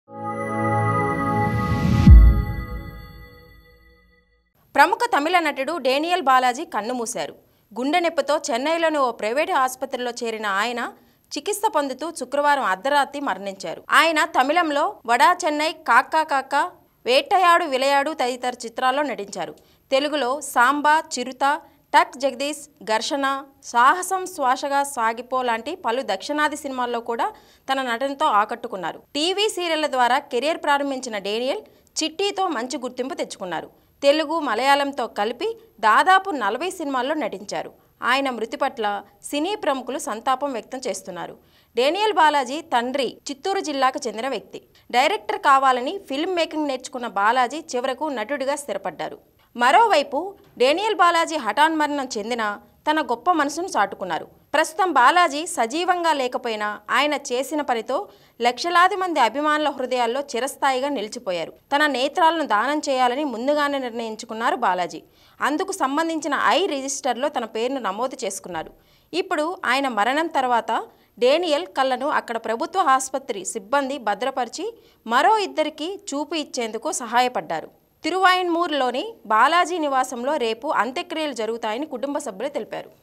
Actor Daniel Balaji Donated His Eyes After His De*th | Telugu Movies | Mirror TV Tollywood
చనిపోయి వారికి జీవితాన్ని ఇచ్చిన డేనియల్ బాలాజీ | Actor Daniel Balaji Donated His Eyes After His De*th | Telugu Movies | Mirror TV Tollywood